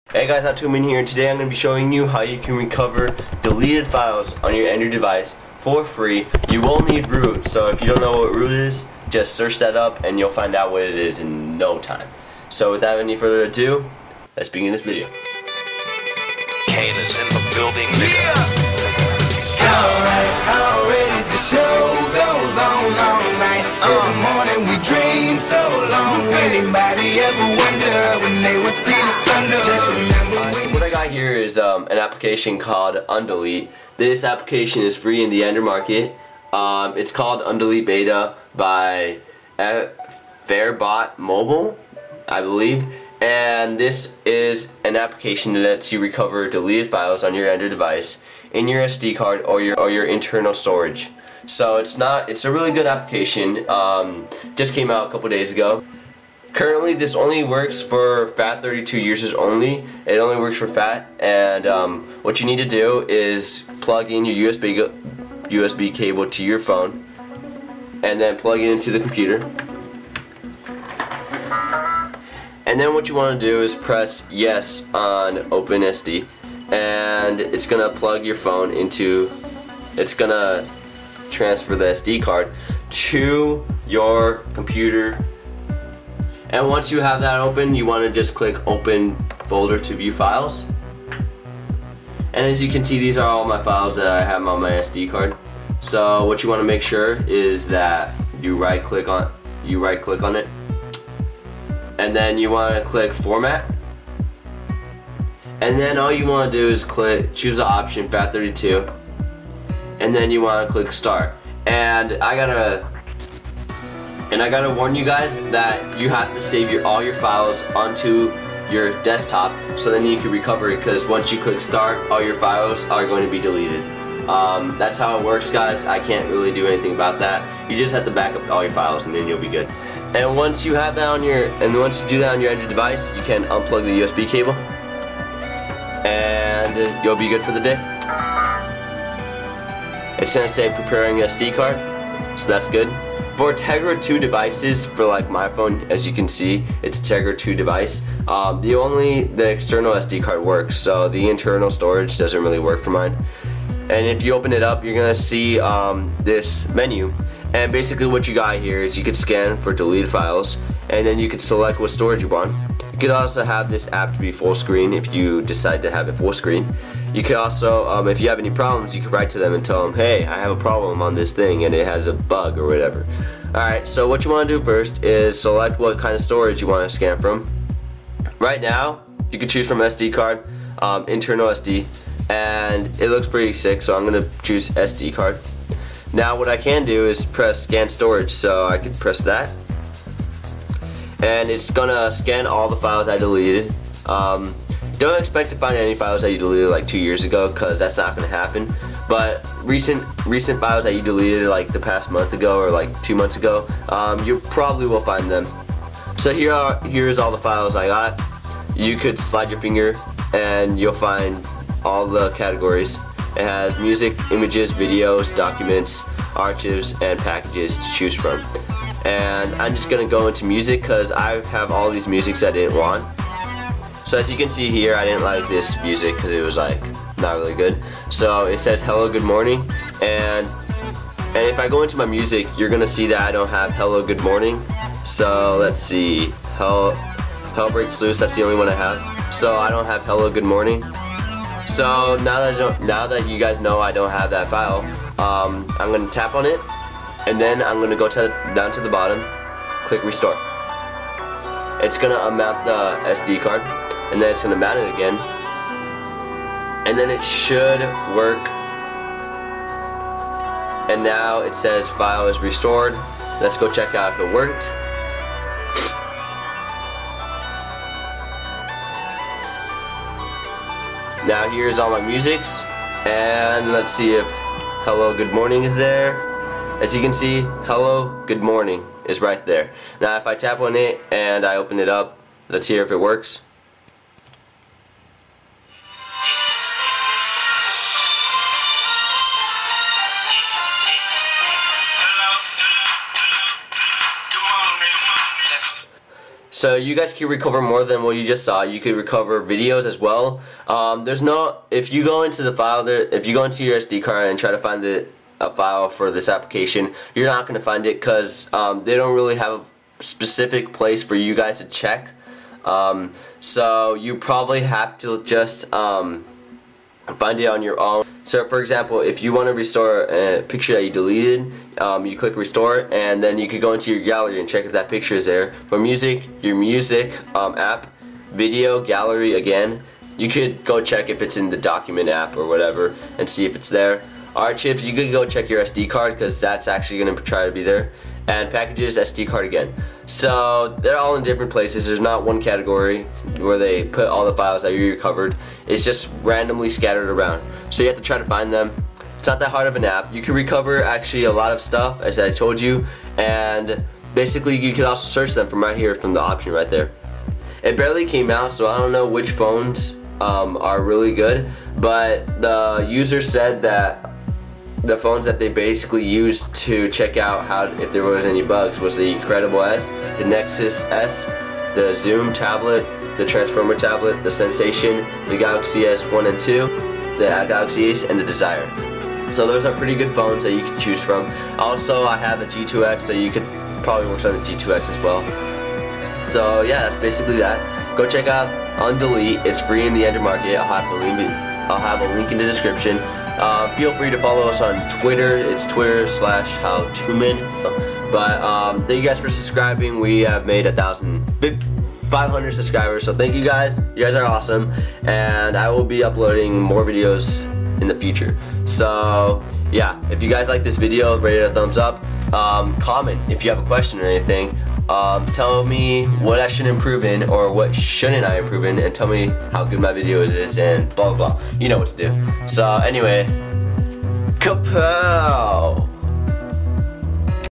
How to recover deleted files on your android device! (Root needed!)
The app that lets you recover deleted files such as images, music, video, documents, archives, and packages is Undeleted Beta!